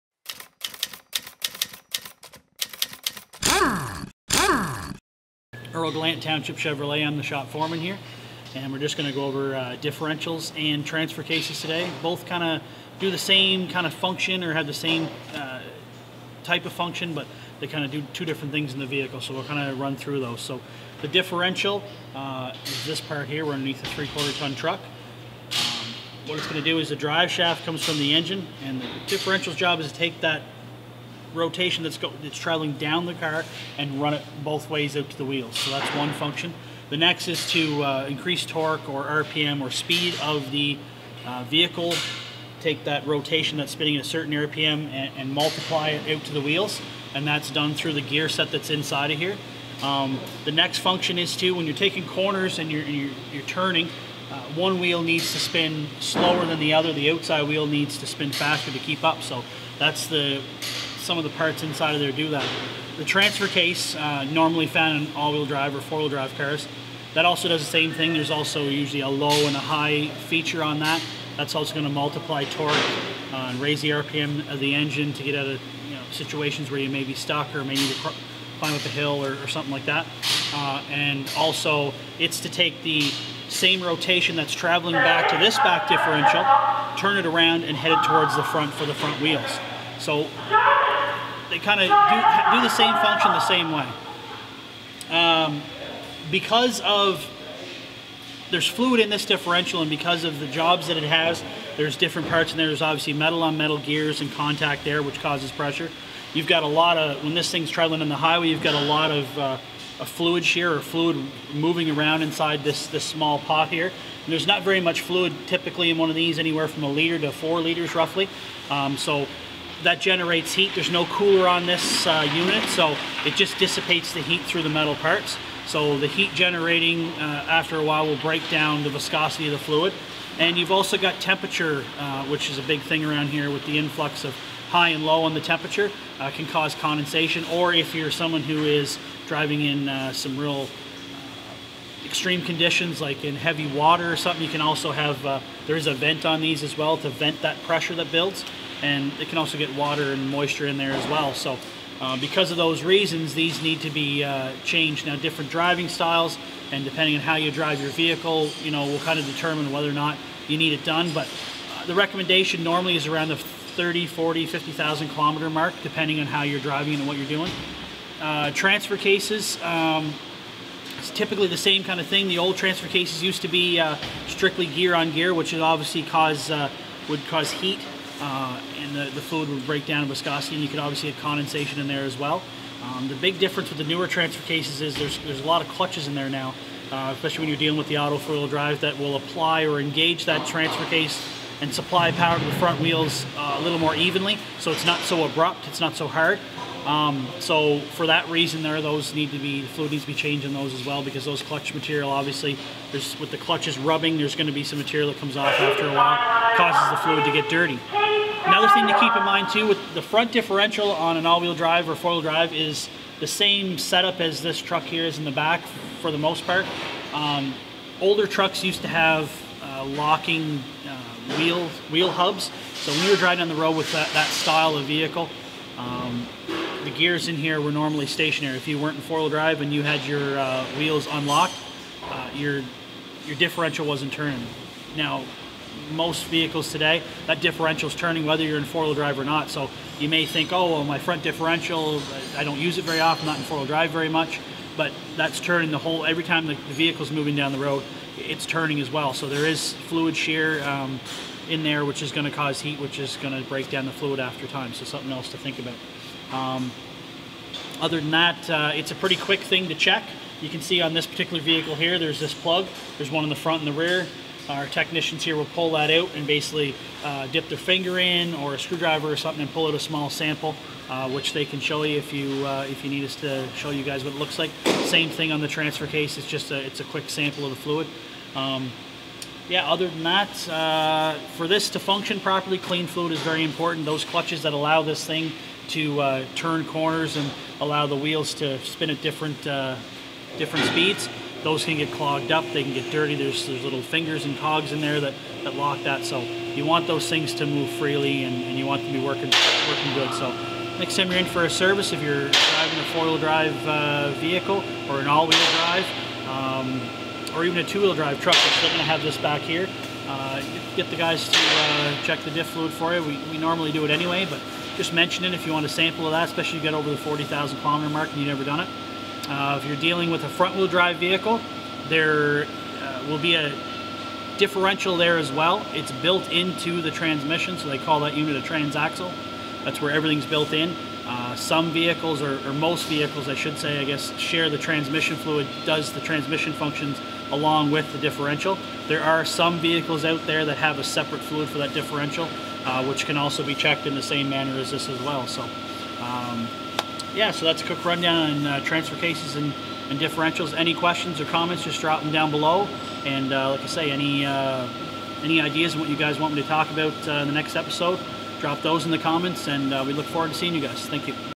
Mechanic Mondays||Tech talk Ep5
Today we learn about Differentials & Transfer Cases! Tune in each week to learn more!